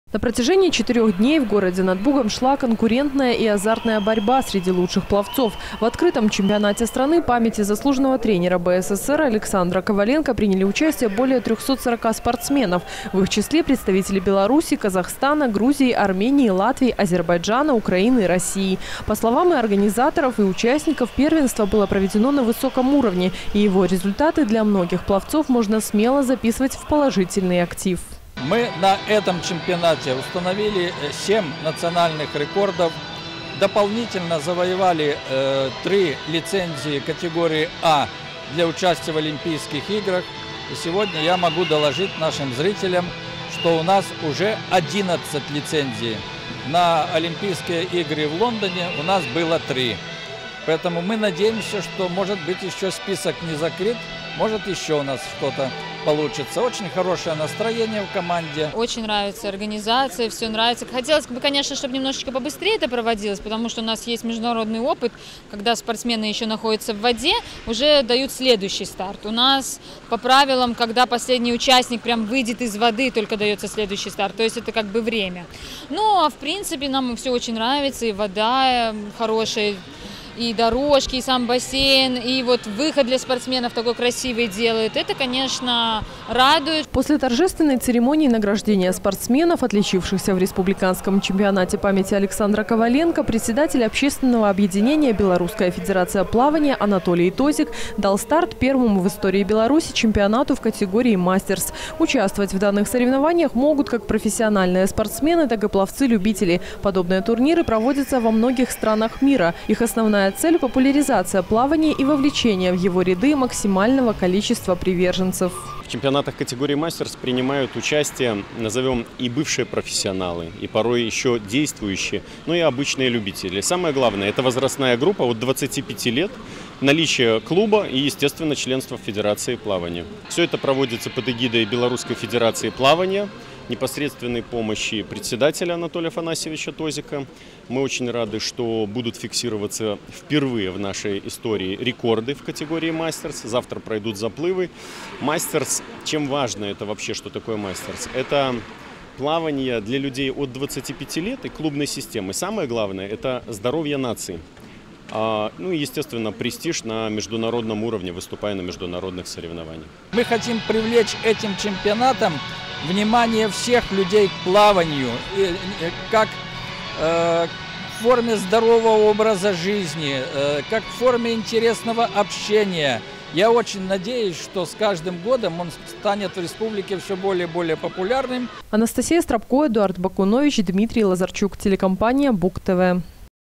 2016-04-27 г. Брест. Чемпионат Беларуси по плаванию памяти Коваленко. Телекомпания Буг-ТВ.
Сразу два первенства по плаванию состоялись в городе над Бугом на минувшей неделе – Брестский областной центр олимпийского резерва по водным видам спорта принимал открытый чемпионат Республики Беларусь памяти Коваленко, а также чемпионат страны в категории «Мастерс». Побывала на первенствах и наша съёмочная группа.
На протяжении 4-ёх дней в городе над Бугом шла конкурентная и азартная борьба среди лучших пловцов. В открытом чемпионате страны памяти Коваленко приняли участие более 340 спортсменов. В их числе представители Беларуси, Казахстана, Грузии, Армении, Латвии, Азербайджана, Украины и России.